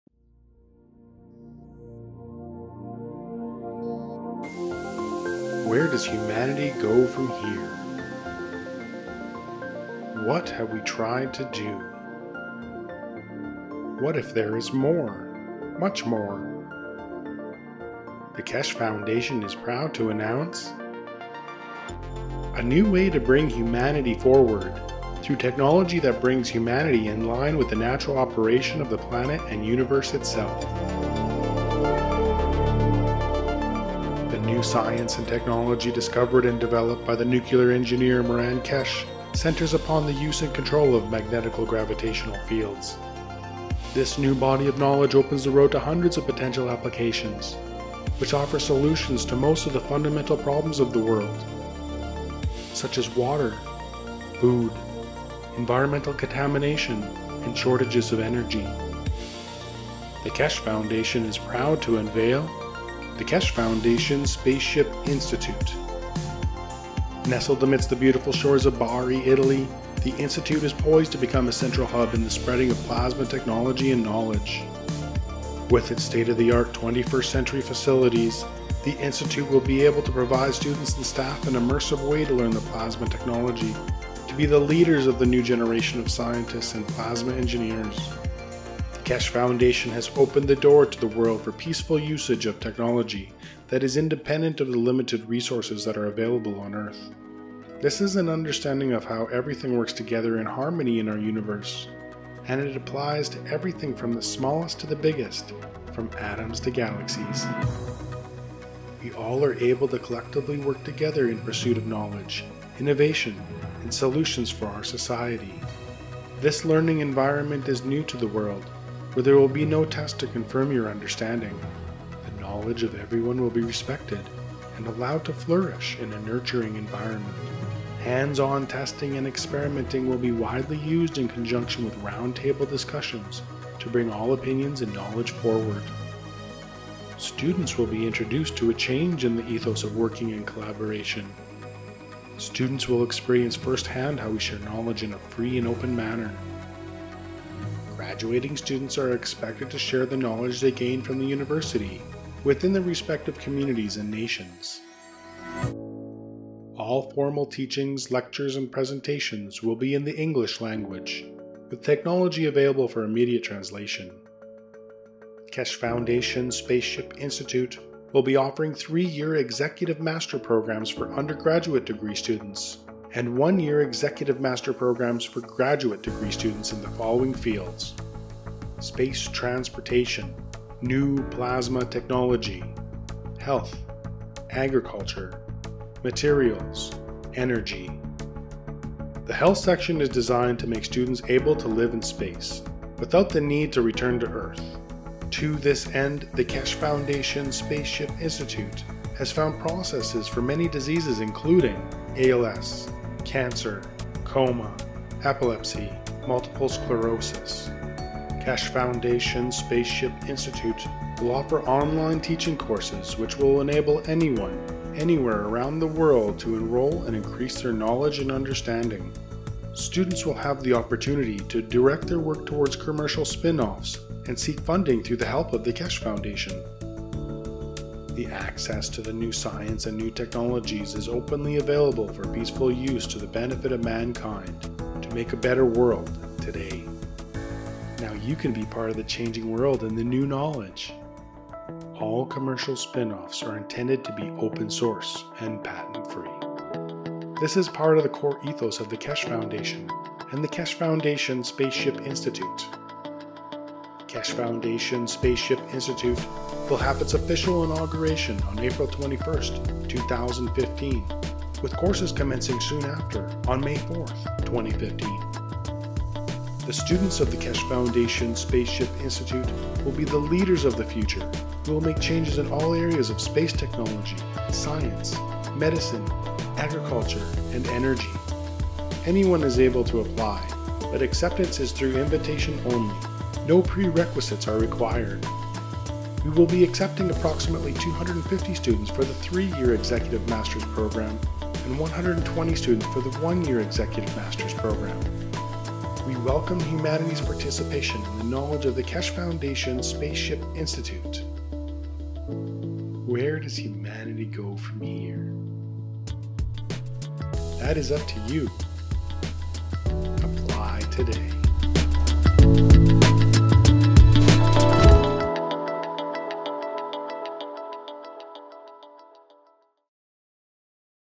KFSSI Promo Video (English with multiple subtitles)
Promo video for the new Keshe school in Italy called,"Keshe Foundation Spaceship Institute" (KFSSI).

The Keshe Foundation Spaceship Institute (KFSSI) is located near Bari, Italy, and will teach the fundamental principles of magnetical-gravitational (magrav) fields as related to lift, new materials, power, and health benefits.

Official opening April 23, 2015.
Classes begin in May 2015.
KFSSI will offer Masters courses in Magrav knowledge and technologies.